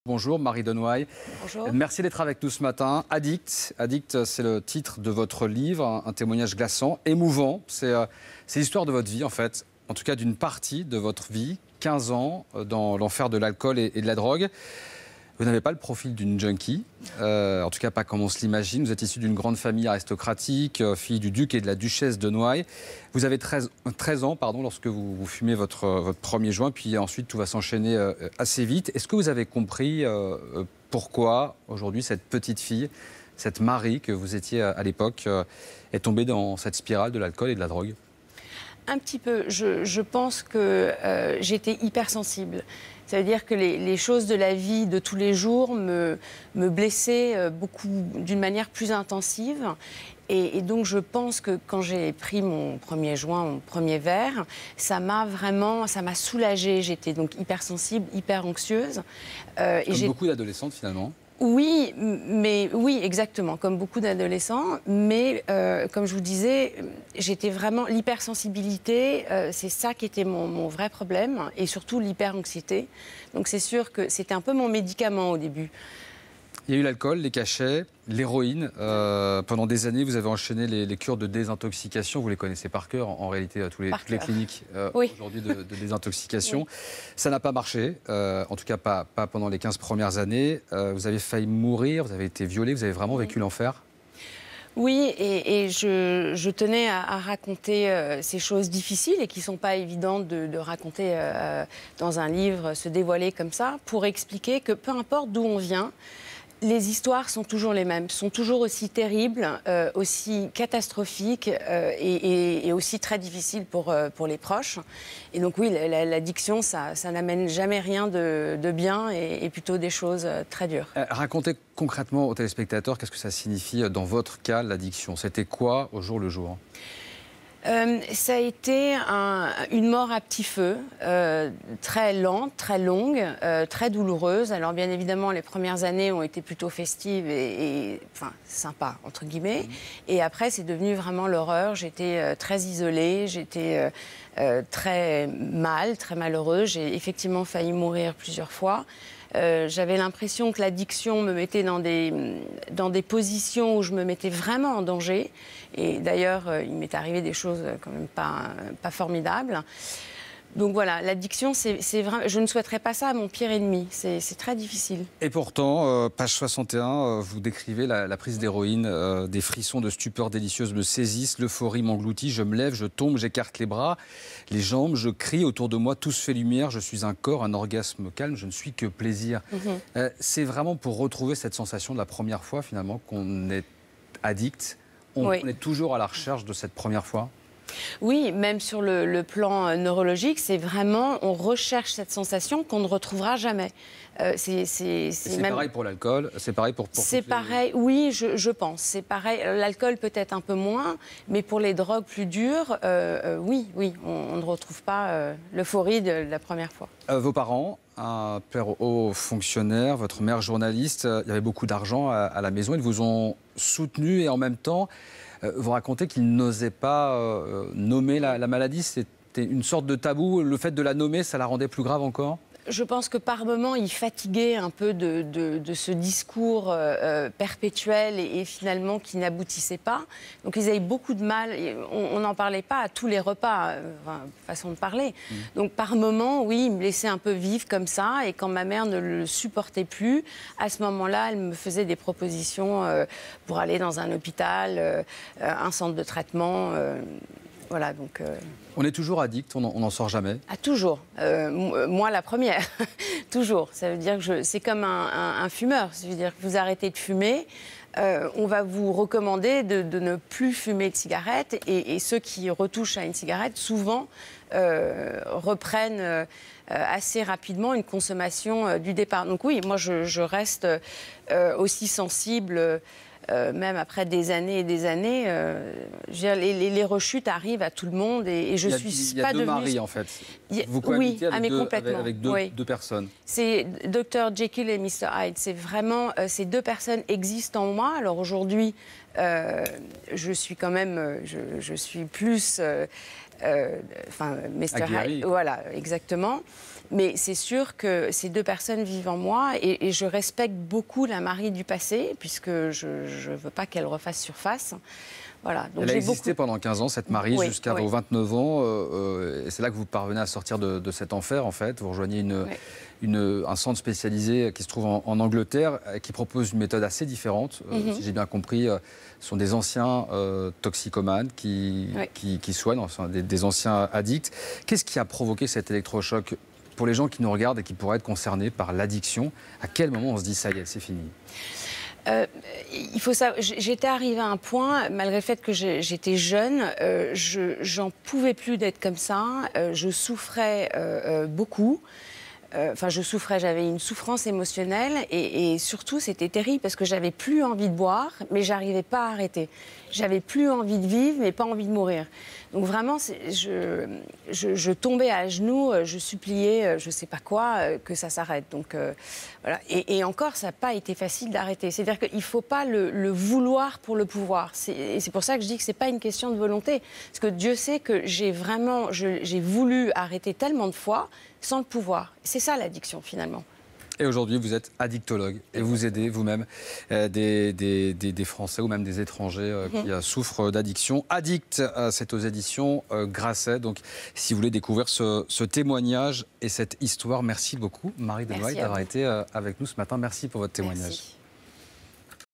Marie de Noailles, ex-addict et addictologue : "ça a été une mort à petit feu. Très lente, ...
Marie de Noailles est une addict. Sevrée depuis plusieurs années, elle soutient qu'on ne devient jamais ex-addict, malgré tout. "Hypersensible", "hyper-anxieuse", elle prend ses premiers "calmants", ses premiers joints, à 13 ans. Suivent les premiers verres, les premiers cachets, jusqu'à l'enfer de l'héroïne. "Peu importe d'où on vient, les histoires sont toujours les mêmes", dit-elle. Les mêmes tragédies, peu importe le milieu. Marie de Noailles raconte "une mort à petit feu. Très lente, très longue, très douloureuse". Aujourd'hui addictologue, diplômée en psychologie de Bristol, elle livre son expérience hors du commun dans un livre, "Addict" (ed. Grasset).